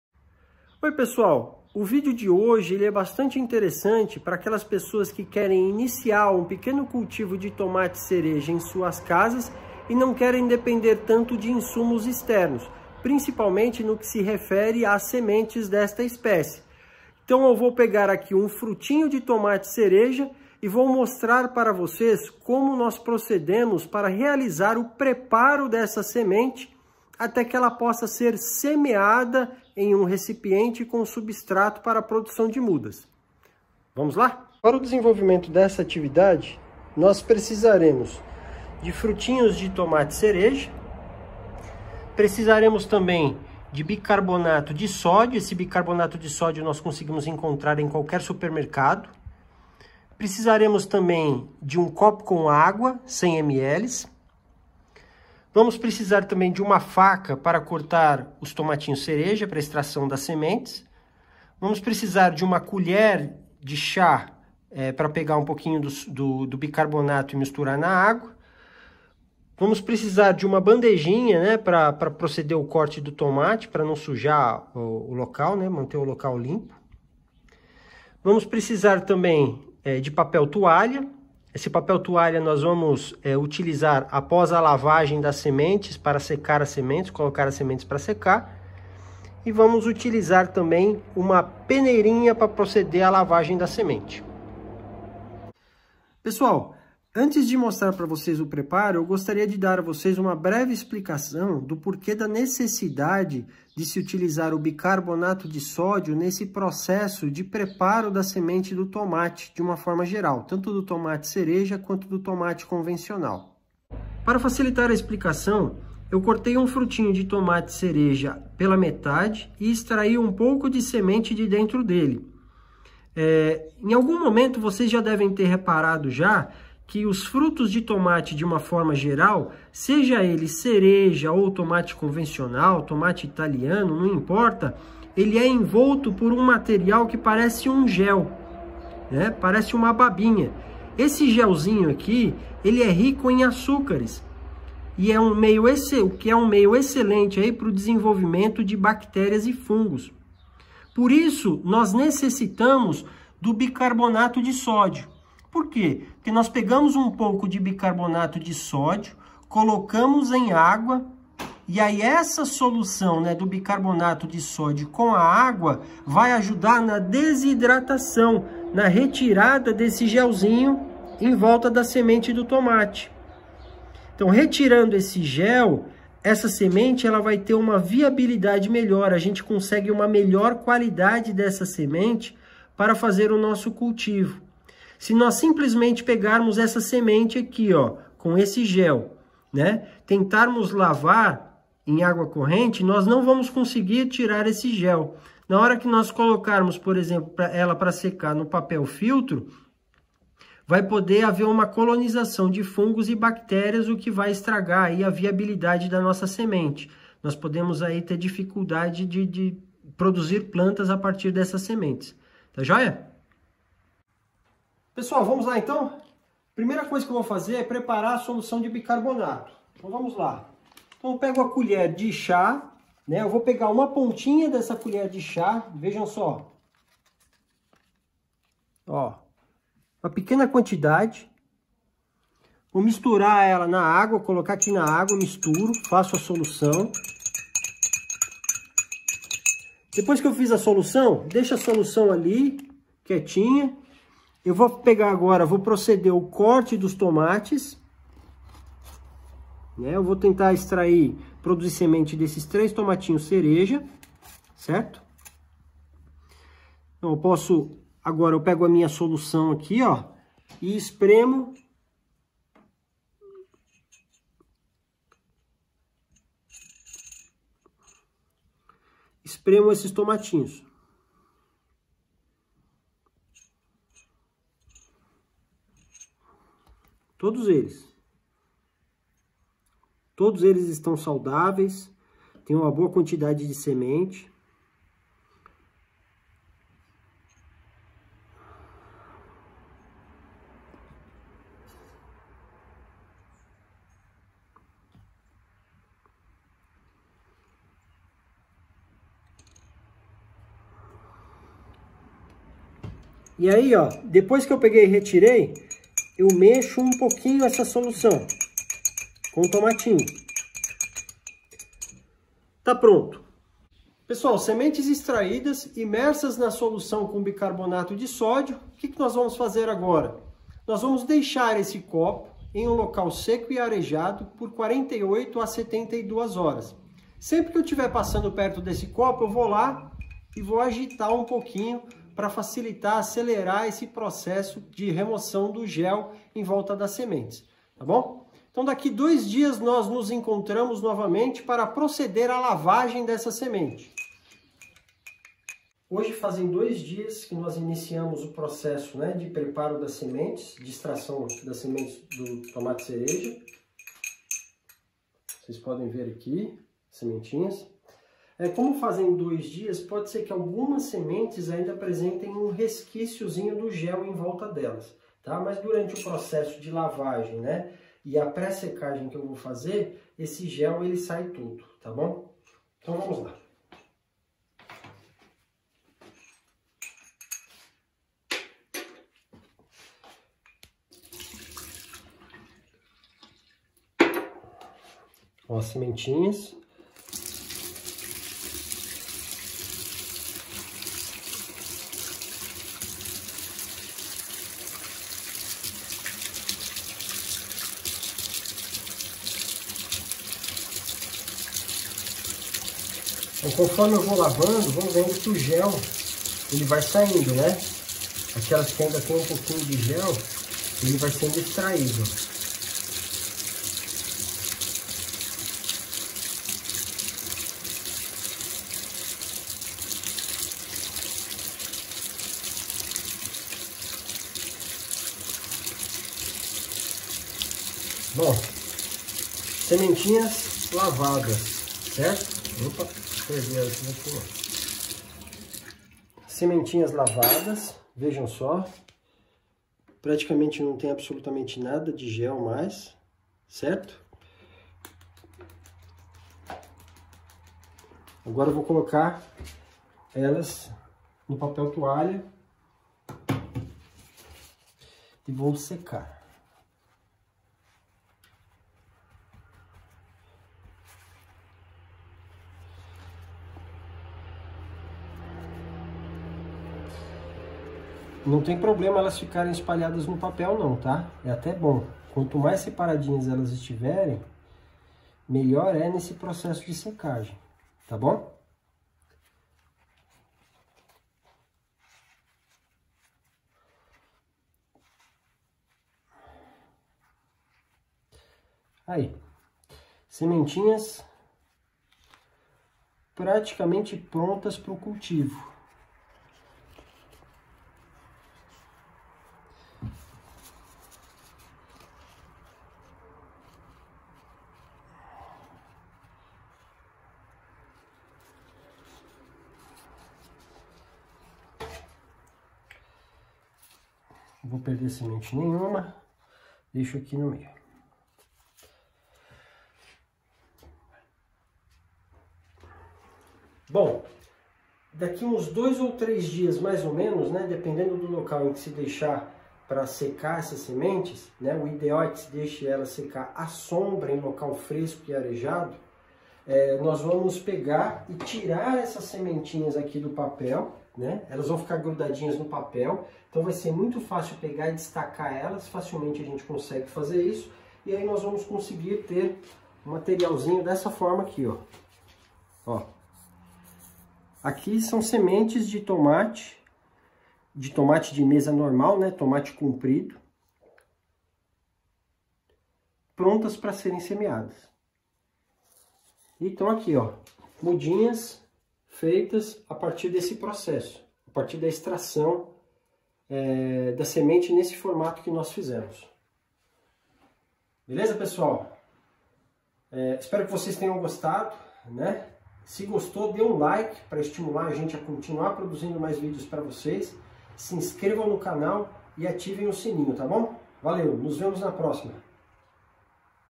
TENHA suas PRÓPRIAS sementes tomate para plantar!!!
No vídeo de hoje mostrarei a vocês como extrair e beneficiar (preparar para o cultivo) sementes de um fruto maduro de tomate. Materiais necessários e procedimentos como retirada do gel (mucilagem que envolve cada semente), lavagem e secagem das sementes serão abordados de forma simples e fácil, para que qualquer um possa se tornar independente no preparo de suas sementes. Esta prática pode facilitar muito o cultivo em locais mais afastados onde insumos, como sementes, sejam mais escassos. Bora lá aprender!?

Nos siga no Instagram: @provaioverde_prof.marcelo

Sementes para plantio de tomate
Como extrair sementes de tomate